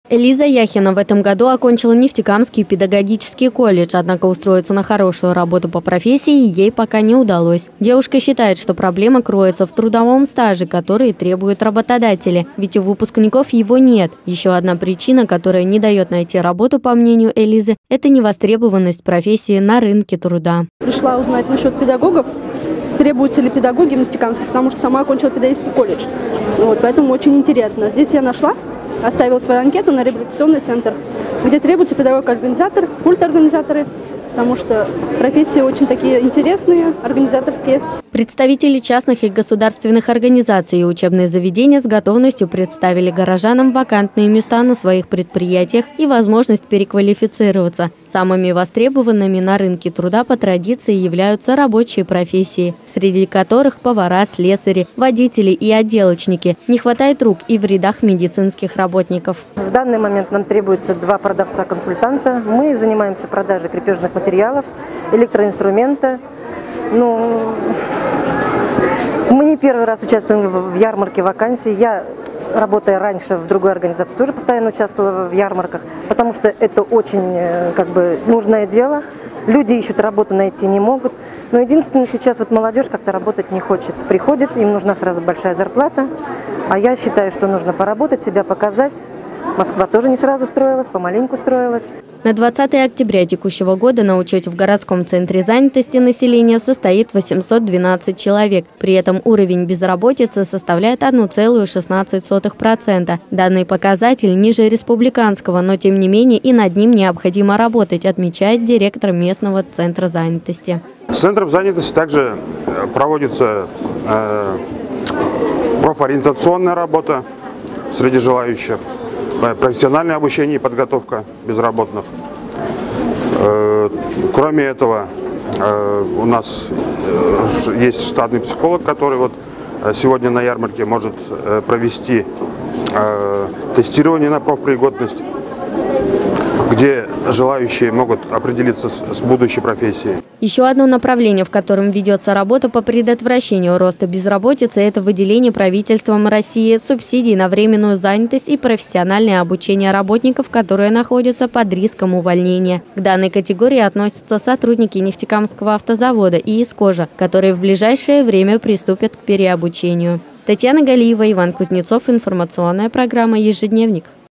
В городе прошла большая ярмарка вакансий
Шанс найти работу появился у жителей и гостей Нефтекамска в среду. Во Дворце молодежи прошла ярмарка вакансий, где работодатели предложили 150 вакансий по различным профессиям.